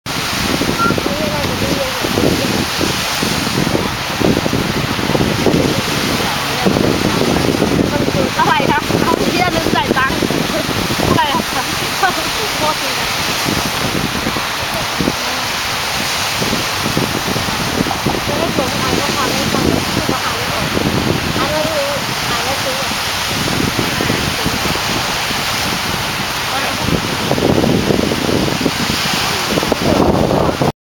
phil walking down jomtien
cloudy day on jomtien beach in pattaya, thailand